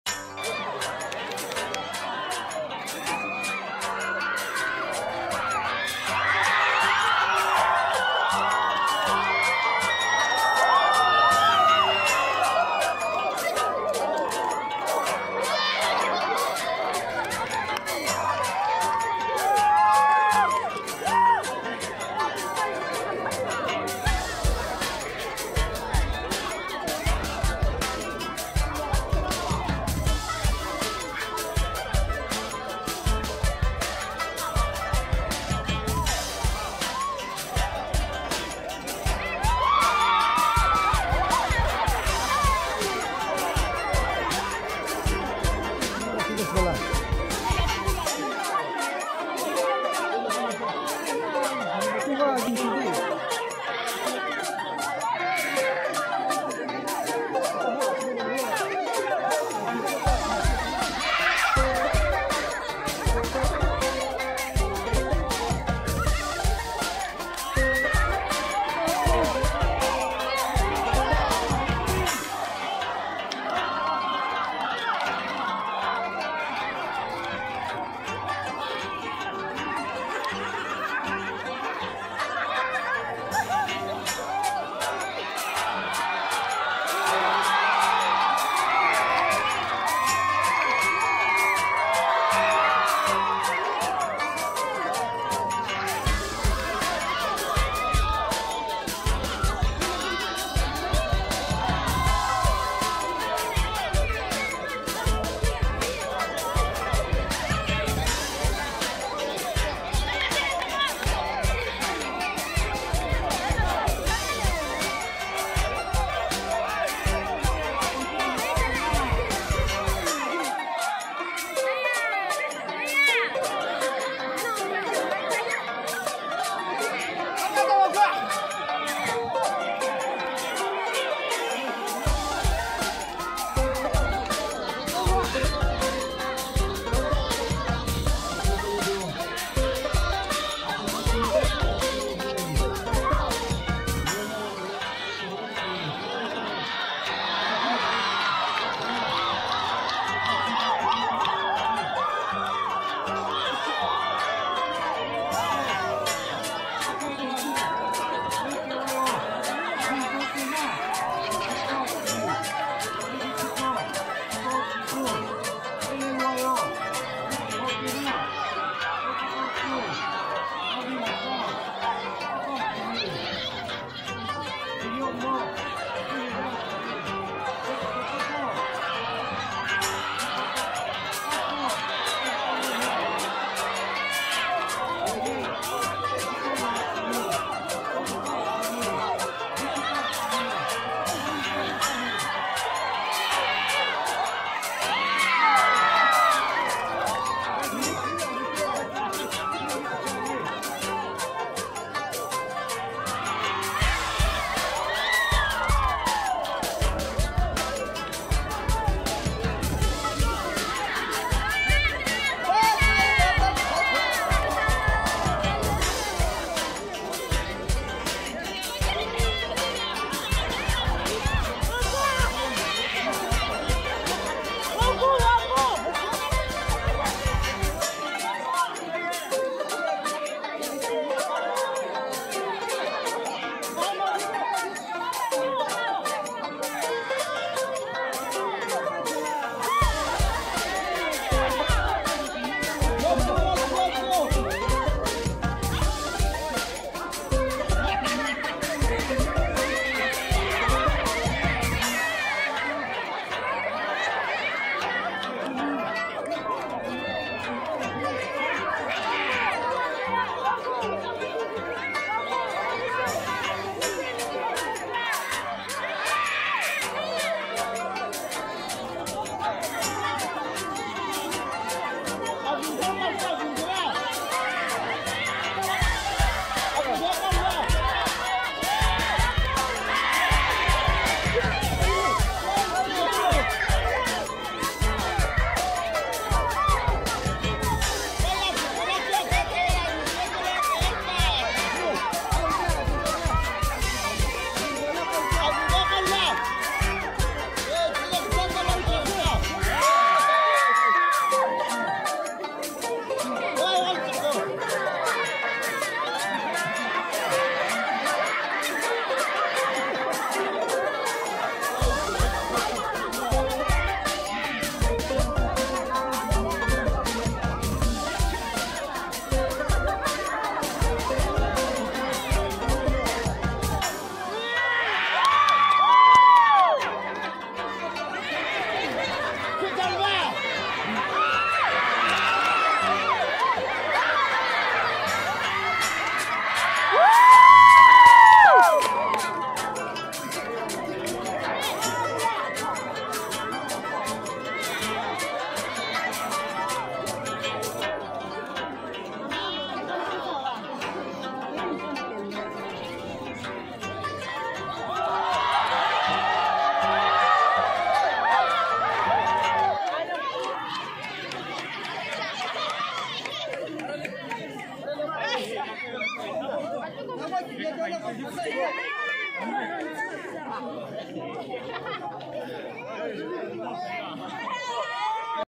TRADITIONAL GREASED POLE CLIMBING° LAZAMI VILLAGE° NAGALAND° NORTHEASTINDIA
The above video was taken from Lazami Students Union 74th Annual Sports meet in Lazami village.
It showcased the strength of both genders equally in a traditional way.